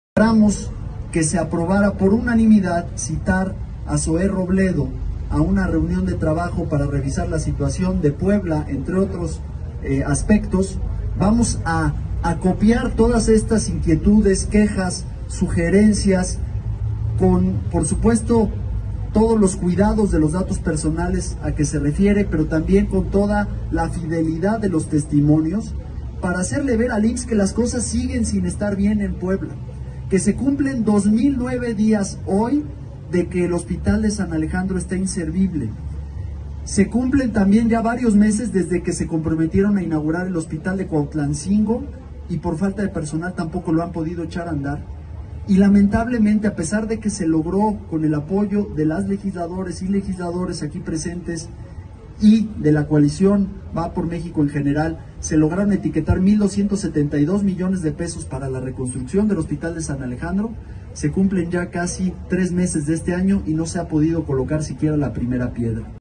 Diputados federales del PAN presentan un módulo de “contraloría ciudadana”
Diputados federales del PAN presentan un módulo de “contraloría ciudadana” para que derechohabientes registren quejas sobre los deficientes servicios del IMSS de La Margarita, en Puebla.

Presentarán las quejas a Zoé Robledo.